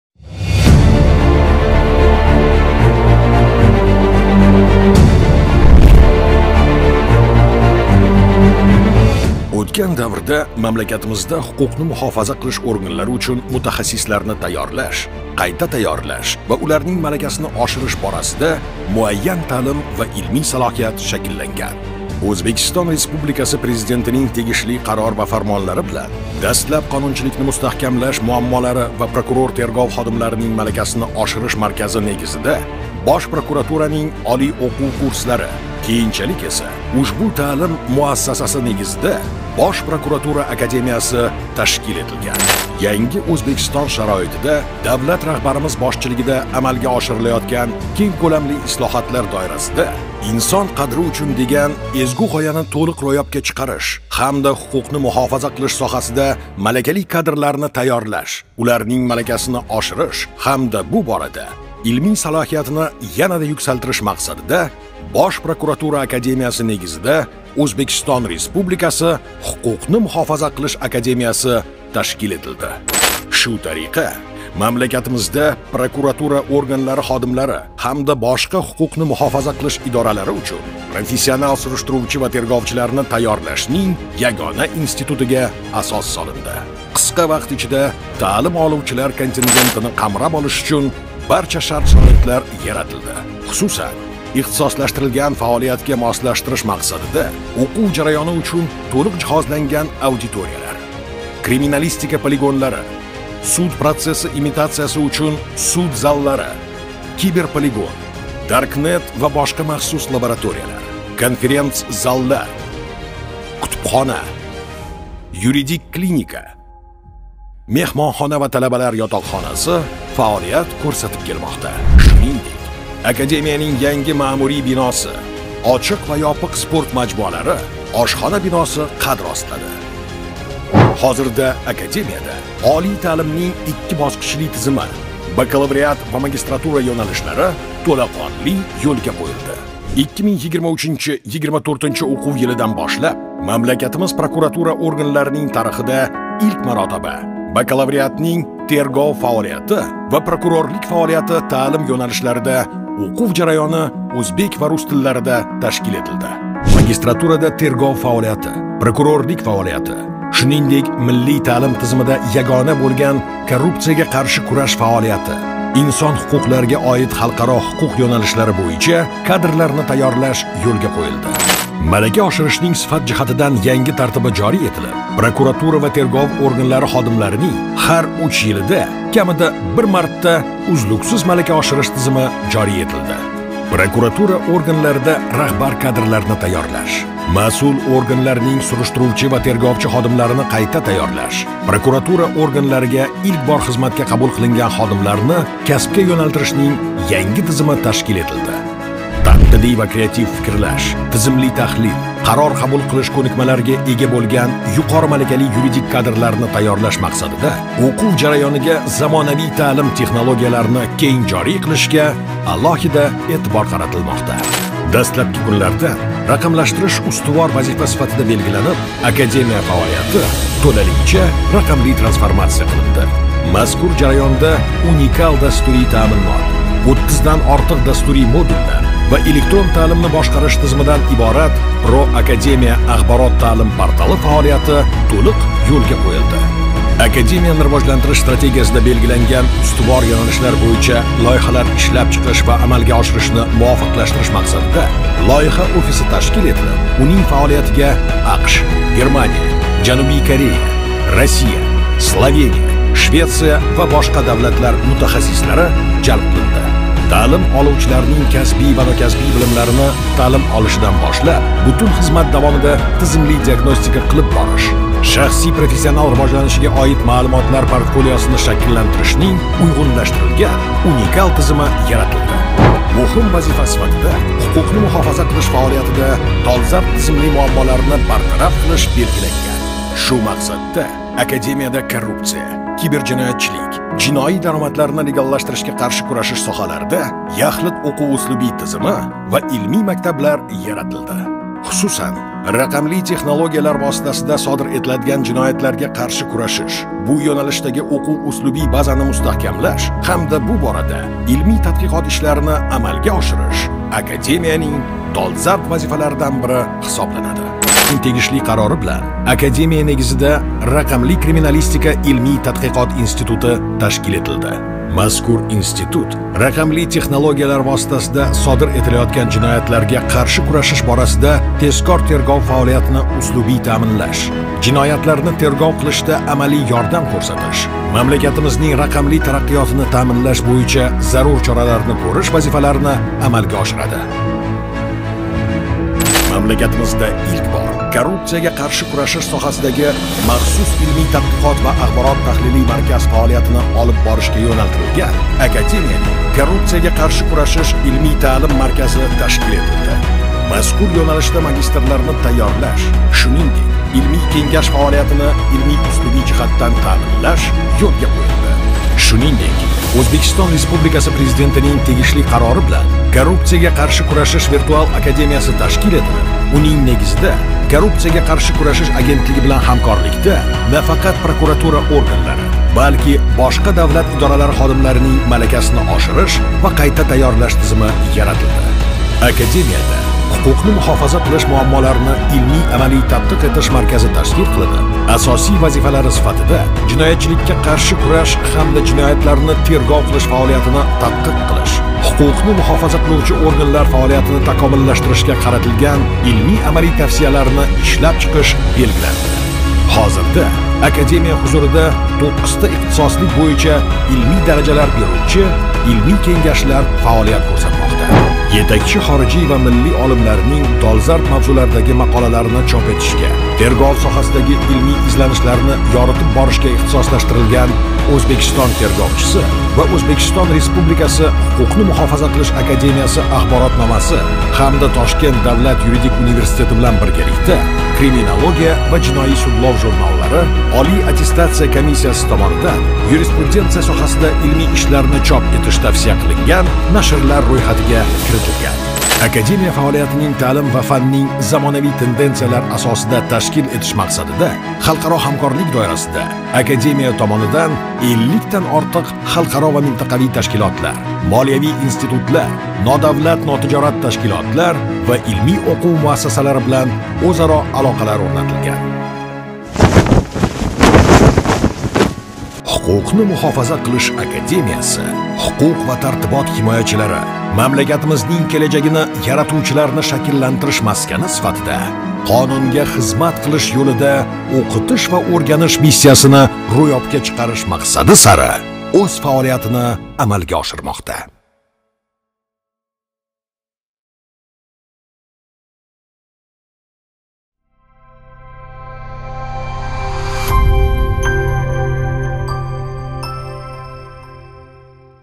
Huquqni muhofaza qilish akademiyasi tashkil etilganining ikki yilligi
Ҳуқуқни муҳофаза қилиш академияси ташкил этилганининг икки йиллиги

Ўзбекистон Республикаси Ҳуқуқни муҳофаза қилиш академияси Ўзбекистон Республикаси Президентининг 2022 йил 28 ноябрдаги “Жиноятларни тергов қилиш соҳасида малакали кадрларни тайёрлашнинг сифат жиҳатидан янги тизимини жорий этиш тўғрисида”ги ПФ–257-сон Фармонига асосан Бош прокуратура Академиясининг негизида ташкил этилган.
Академия “Турли тизимлар – ягона ёндашув” тамойили бўйича суриштирув ва дастлабки терговни амалга оширишга ваколатли давлат органлари учун малакали кадрлар тайёрлайдиган ихтисослаштирилган олий таълим ва илмий-тадқиқот муассасаси ҳисобланади.
Қисқа муддат ичида Академияда таълим, илмий-тадқиқот ва эксперт-таҳлил соҳаларининг барча йўналишларида белгиланган вазифаларни тўлиқ амалга ошириш учун зарур салоҳият шакллантирилди.
Академия жамоаси ўз фаолиятини “Қонунга хизмат қилиш йўлида ўқитиш ва ўрганиш” («Et docere et discere servitute legis») шиорига содиқ ҳолда ташкил этадилар ва амалга оширадилар.